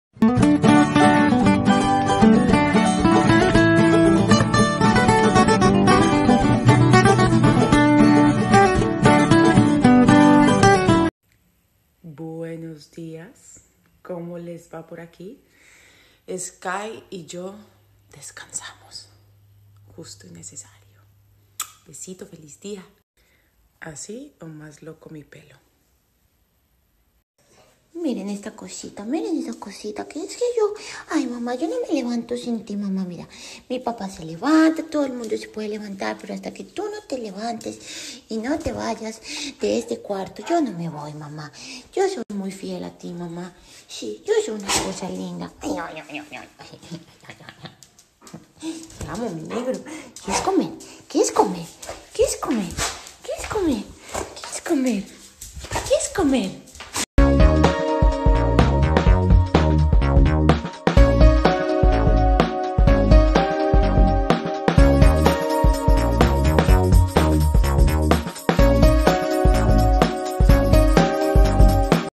Ximena Duque, Skye y ella descansaron mucho - Muestra tal cómo se despierta
Ximena Duque nos cuenta que descansó mucho con Skye y que oreo  se levanta hasta que ella sale de su cuarto es muy fiel a ella